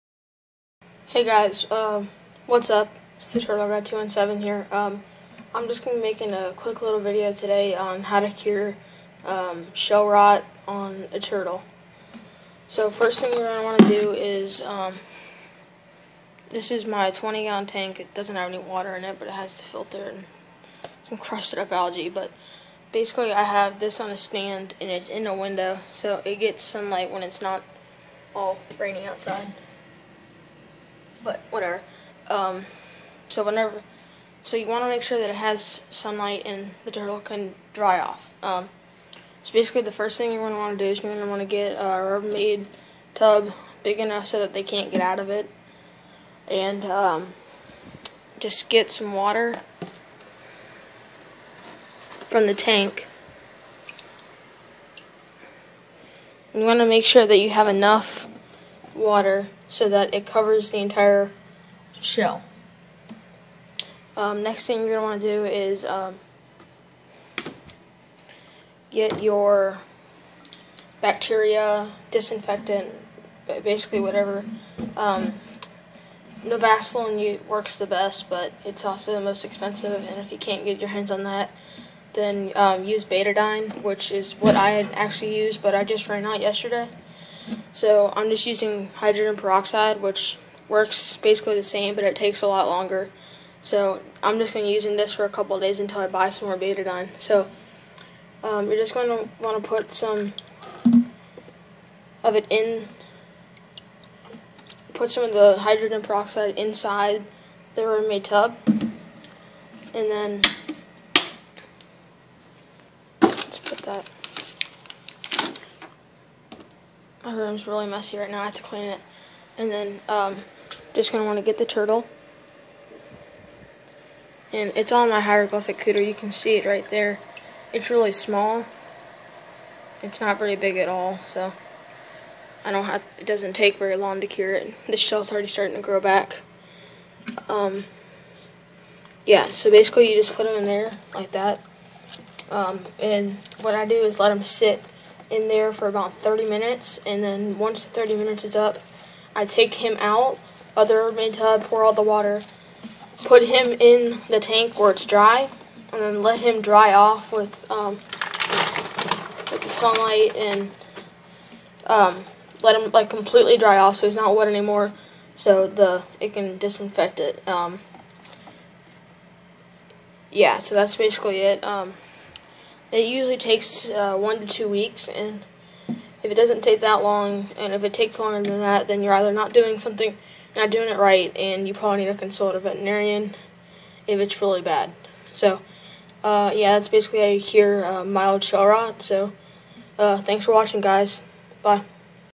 How to cure shell rot
Hey this us a simple description on hiw to cure shell rot. It will usually take 1-2 weeks to heal